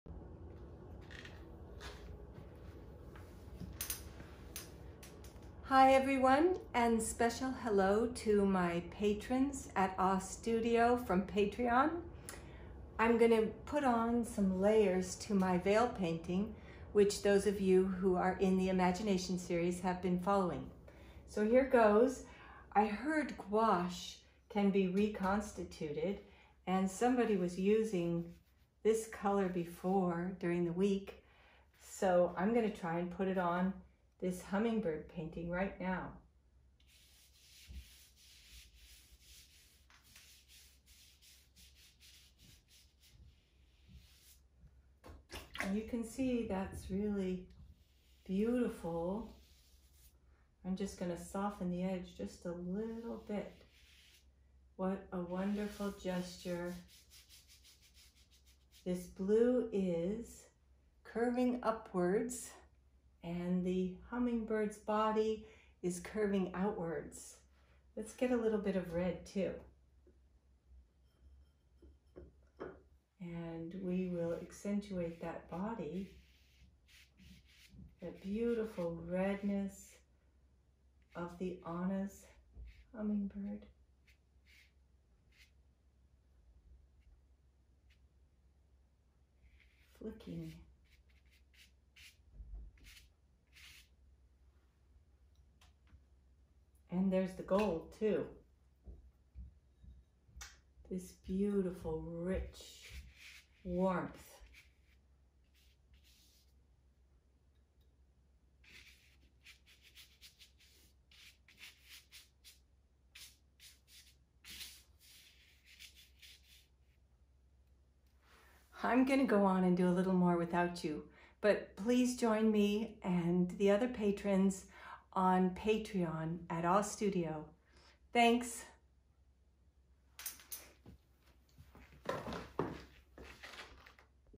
Veil Painting Demonstration
Watch me demonstrate Veil Painting for 2 minutes and if you like it please join